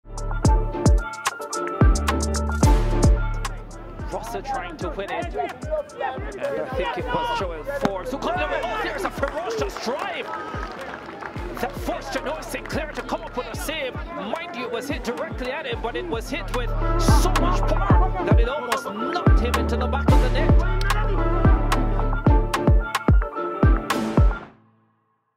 Johnoy Sinclar top save for May Day High vs DeCarteret College is the save of the week for Week 6!
SportsMax presents the save of the week for Week 6.